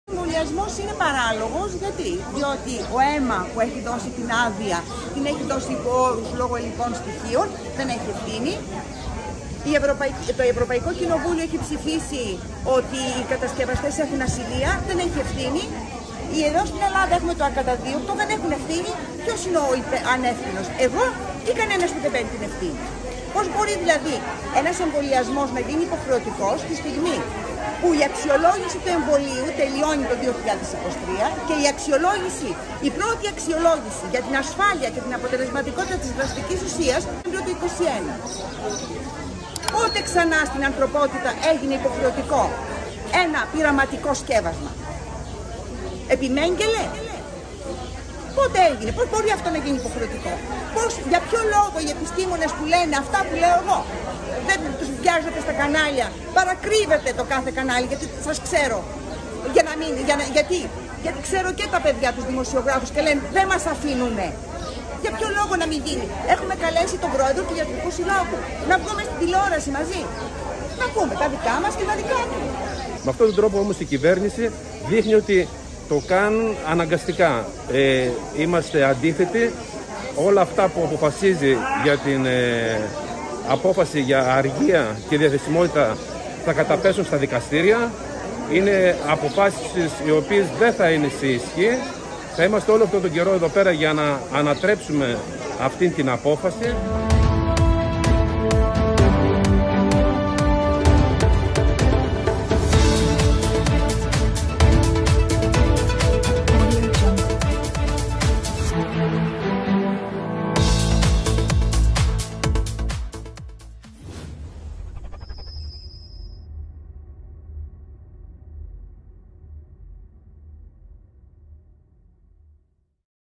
GEOFUN NET RADIO SPOT
Ξεσηκώθηκαν οι υγειονομικοί στην Λάρισα αλλά δεν στο έδειξαν πουθενά ! Από σήμερα σε αναστολή